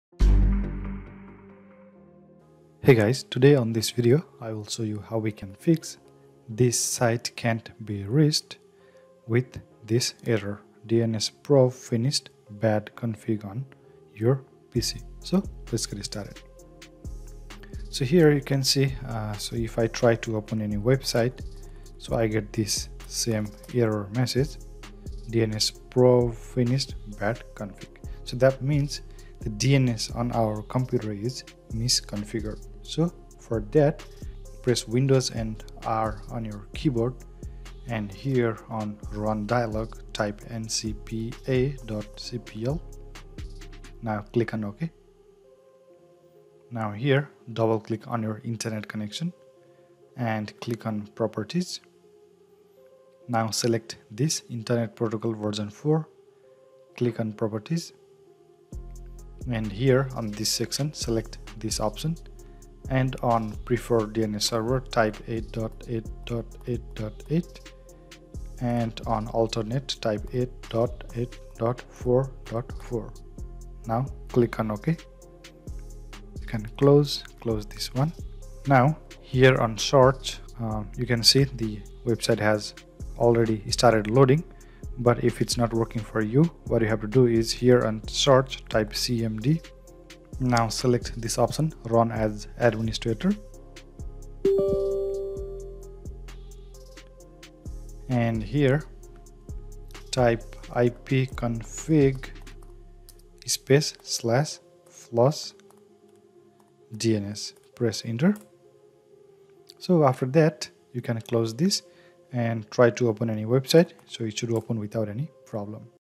How To Fix: This Site Can't Be Reached, Google Chrome, Windows 10 | DNS PROBE FINISHED BAD CONFIG
This site can't be reached error on google chrome with error message DNS_PROBE_FINISHED_BAD_CONFIG is caused on Windows 10 PC because of bad DNS configuration. If your DNS server is unable to resolve the IP address of the domain name that you are trying to visit then you will get this error message.

This problem can simply be solved by changing the DNS configuration of your computer. On this video, I will show you how we can fix the problem of This site can't be reached on google chrome with error message DNS PROBE FINISHED BAD Config on Windows PC.

This tutorial will work on any version of Windows, so if you are using Windows 10, Windows 7 or Windows 8, you can apply this solution to fix the problem of this site cannot be reached.